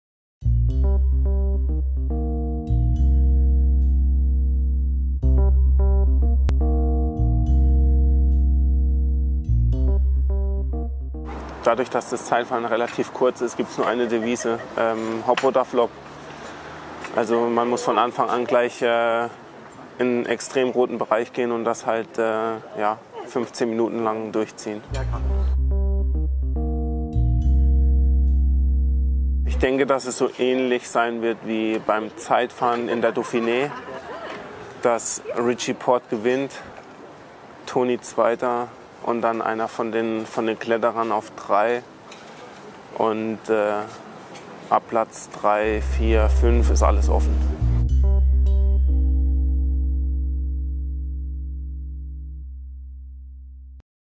Favoritencheck für den Prolog der Tour de France 2017 von Jasha Sütterlin
Von Anfang an Vollgas!"

Die Spannung steigt! Um 15:15 Uhr startet die 104. Tour de France in Paris. Insgesamt sind 16 Fahrer aus Deutschland dabei. Um 17:46 Uhr wird Jasha Sütterlin in seine erste Tour de France gehen. Er ist ein starker Zeitfahrer! Wie er in das Rennen gehen wird, erfahrt ihr im Interview!

Welche Reihenfolge wird es im Ziel wohl geben?

RennRad Jasha Sütterlin Fans

Hier die Startzeiten der Deutschen Teilnehmer:

15:25 Rüdiger Selig (Bora-Hansgrohe)//15:44 Nikias Arndt (Sunweb)//15:47 Emanuel Buchmann (Bora-hansgrohe)//15:58 Christian Knees (Sky)//16:04 Paul Martens (LottoNL-Jumbo)//16:26 Robert Wagner (LottoNL-Jumbo)//16:30 Rick Zabel (Katusha-Alpecin)//17:01 John Degenkolb (Trek-Segafredo)//17:35 Marcel Sieberg (Lotto-Soudal)//17:36 Nils Politt (Katusha-Alpecin)//17:46 Jasha Sütterlin (Movistar)//17:56 Simon Geschke (Sunweb)//17:59 Marcus Burghardt (Bora-hansgrohe)//18:19 André Greipel (Lotto-Soudal)//18:20 Tony Martin (Katusha Alpecin)//18:22 Marcel Kittel (Quick-Step Floors)